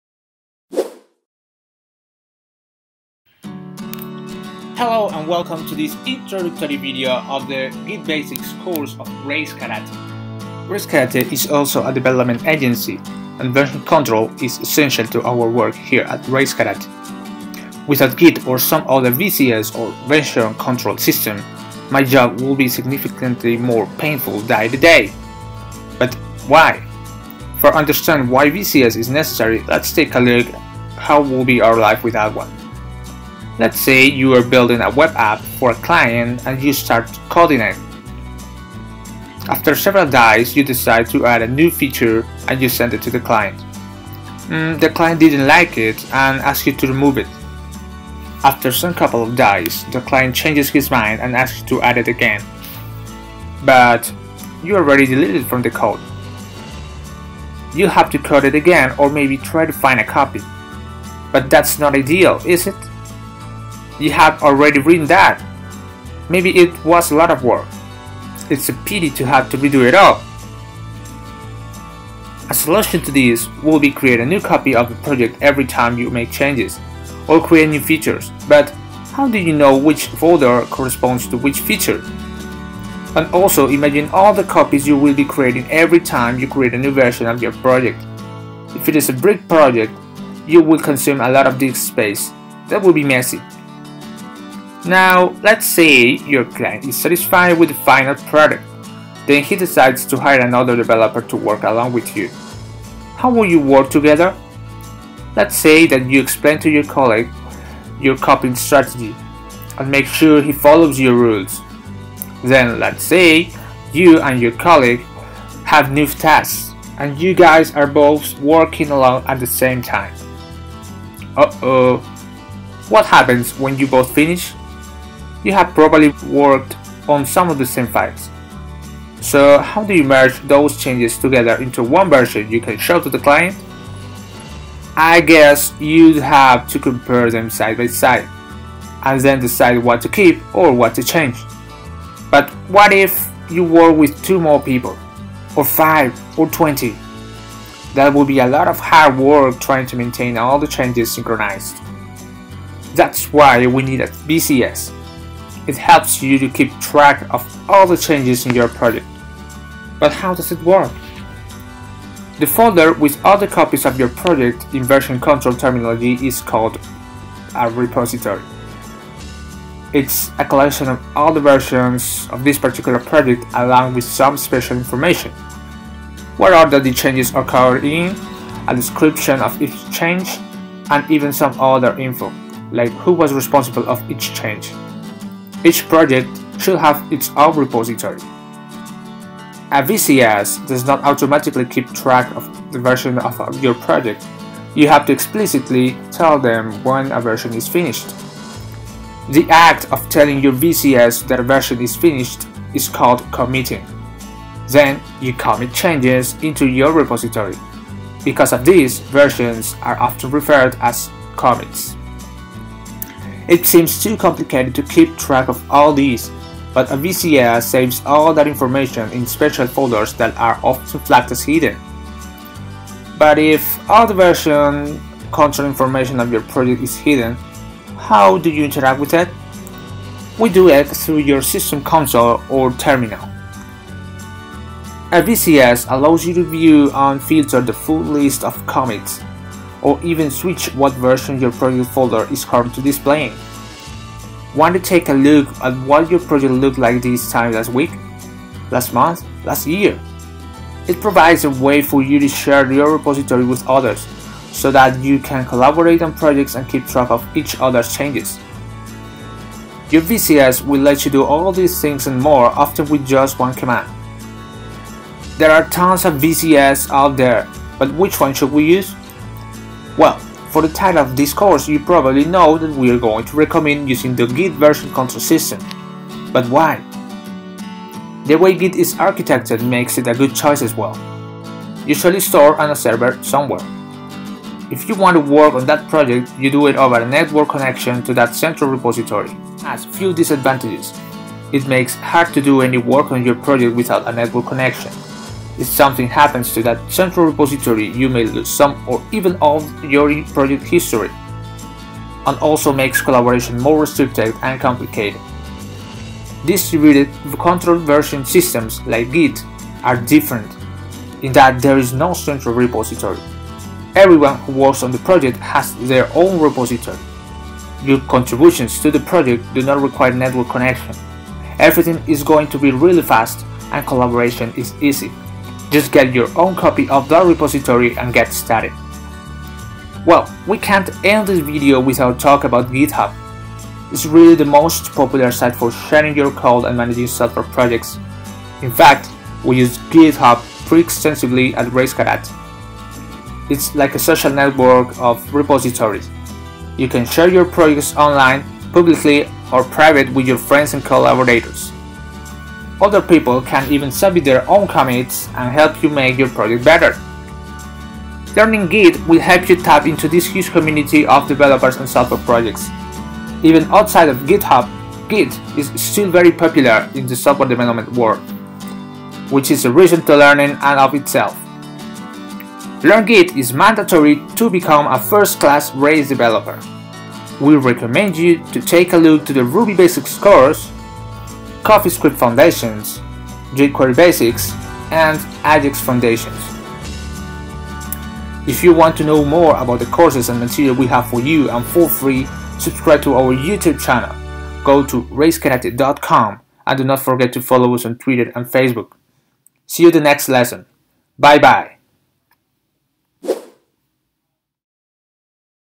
Git Basics course | What is and how it works | Intro
Welcome to this introductory video of the Git Basics course of Rails karate, in this video we'll talk about what is a VCS and why we as developers need to use it, also we'll talk about why choose Git as our VCS and bout Github